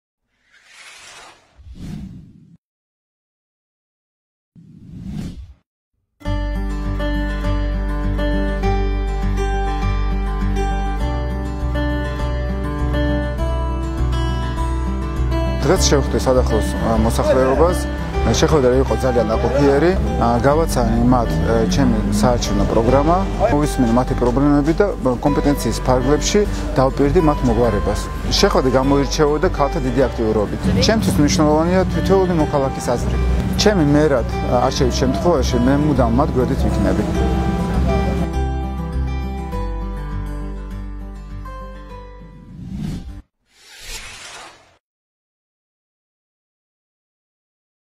Temur Abazov #41 (თემურ აბაზოვი)
შეხვედრა სადახლოს ადმინისტრაციული ერთეულის ამომრჩეველთან